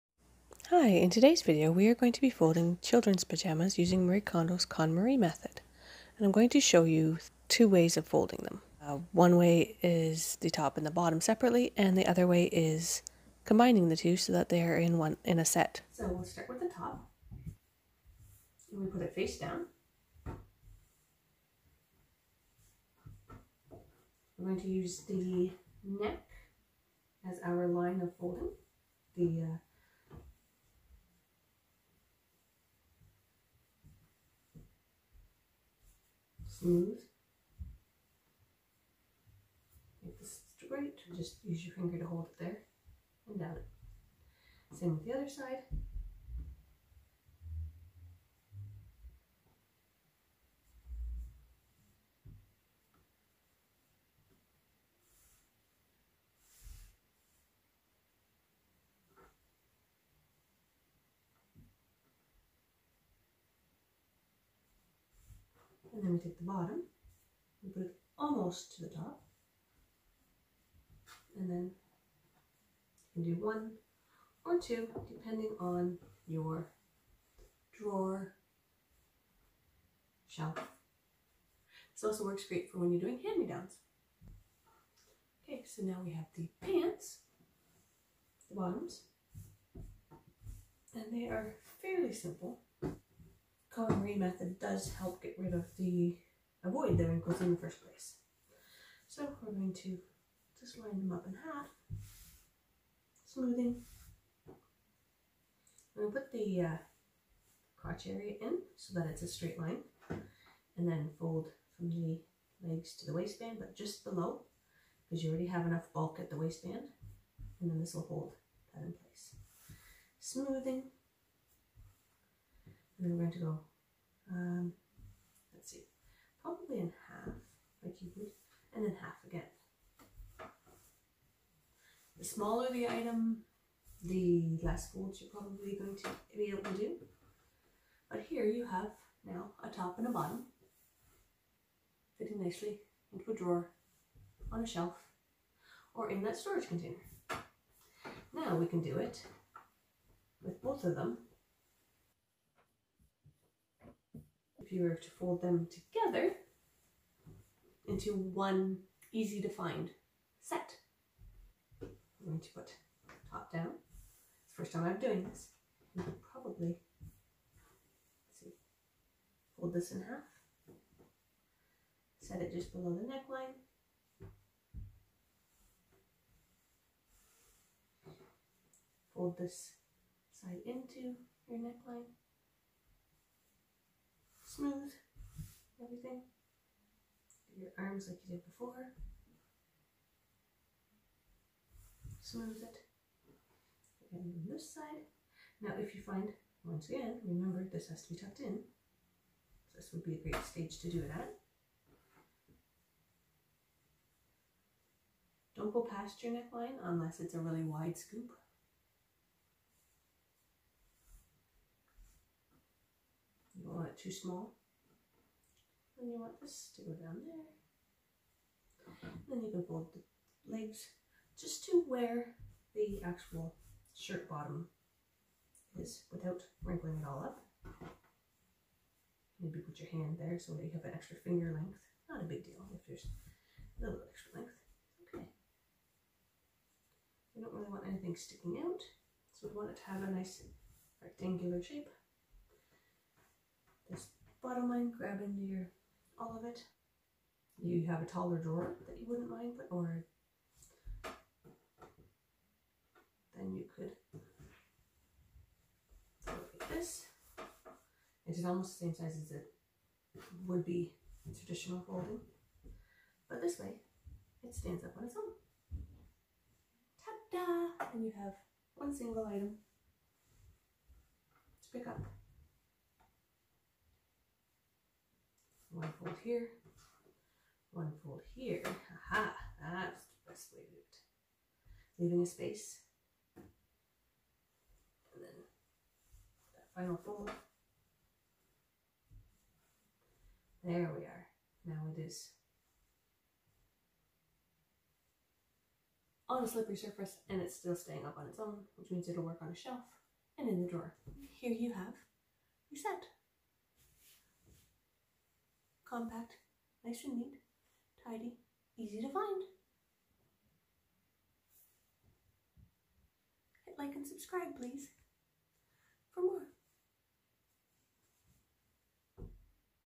konmari fold children's pajamas / kid's pj's. Fold pajama sets into 1 bundle
How to konmari fold children's pajama sets / pj's individually and together into a bundle.
Marie Kondo's konmari method of folding works great for storing items in drawers, on shelves, and even saving space putting into storage to wait as hand-me-downs.

Follow along with this simple video and you will see how easy it is. This way of folding makes it easy to find what you need in a hurry. No more refolding messy drawers or shelves over and over - you no longer make a mess searching through the pile to find what you want.
So easy to keep neat and tidy now.